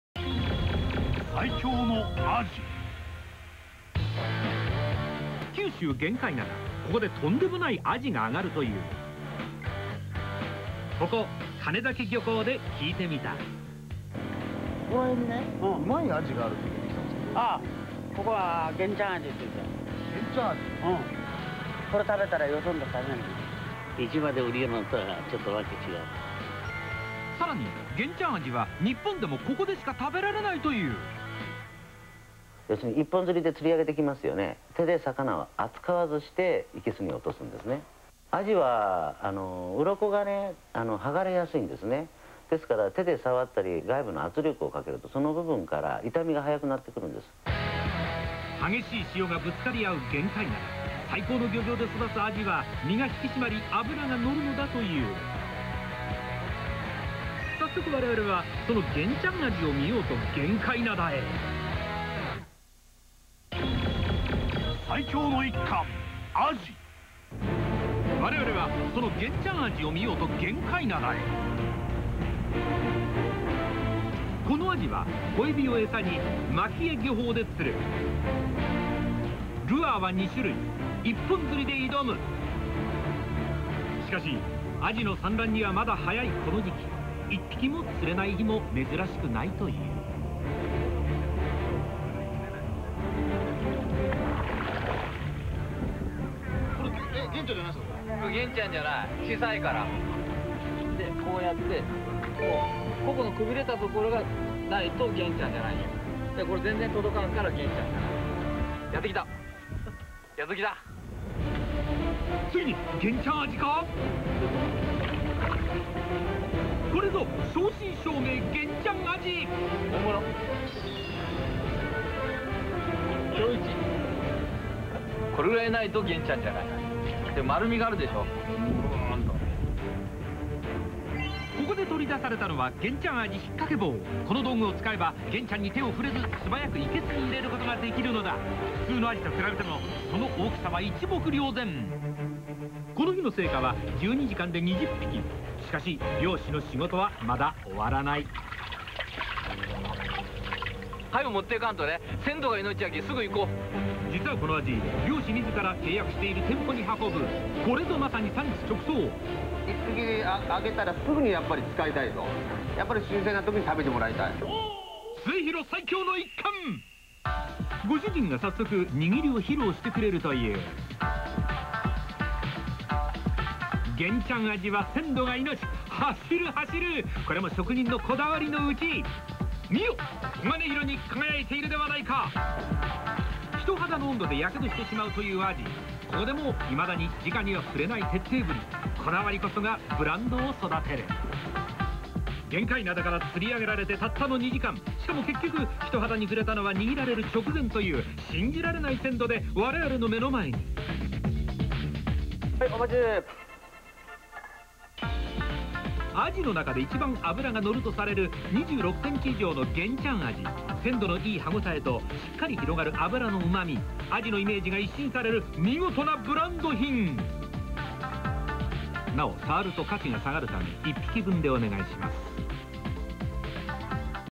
寿司 福岡・宗像 末広
福岡・宗像「末広」です。鐘崎漁港で揚がった玄ちゃんアジは、手で魚を扱わずして生簀に落とし、「新鮮な時に食べてもらいたい」と言って、アジの中で一番脂が乗るという「玄ちゃんアジ」を握る映像です。